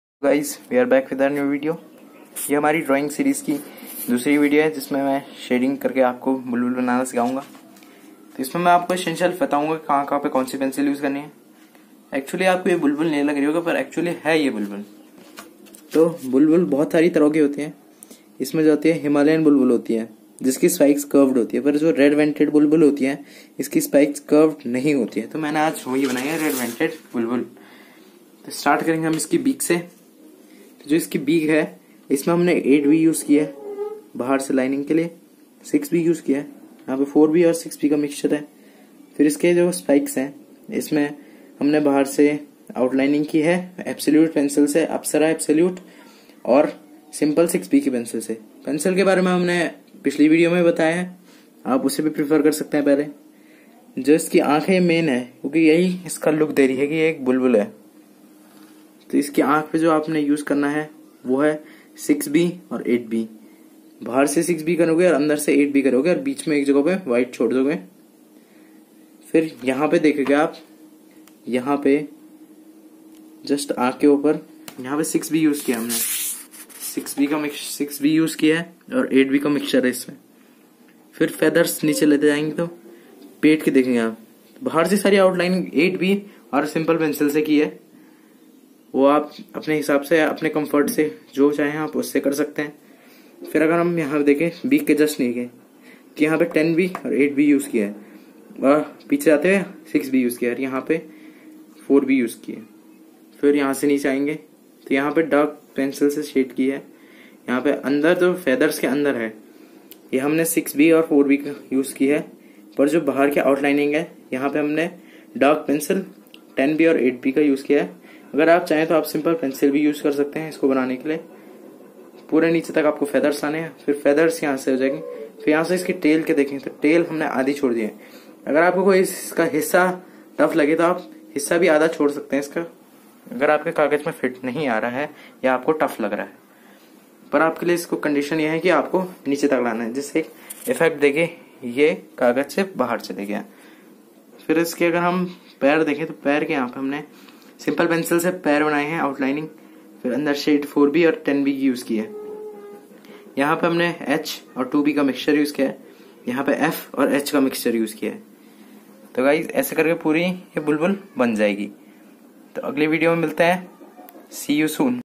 Learn how to make Bulbul
In this video we will make red vented bulbul by telling you about different shades used